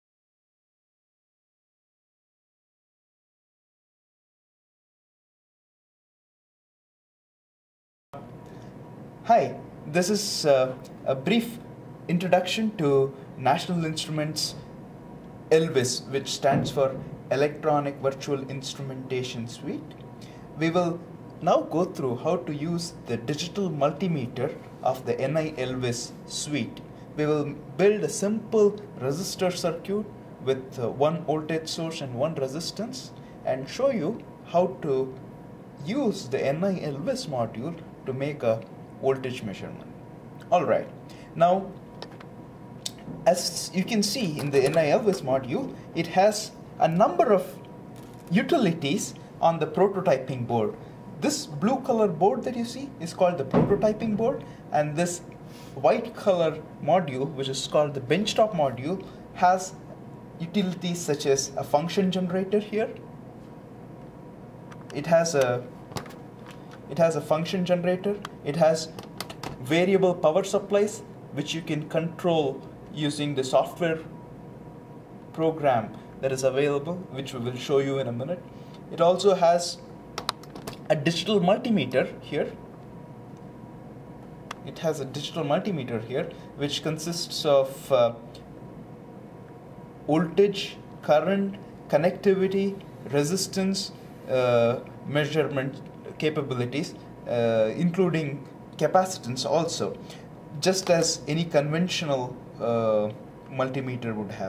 Voltage Measurement using NI ELVIS II - Part 1
This is the first part of a two part series on DC Voltage Measurement using NI ELVIS II module.